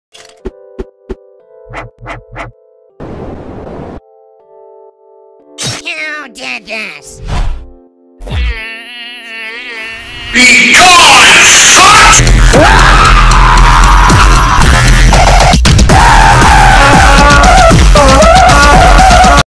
Four says BEGONE !!!!
Animation by Some Mii Gunner. Editing by me! Sorry this idea popped into my head at school today and sorry to headphone users. :3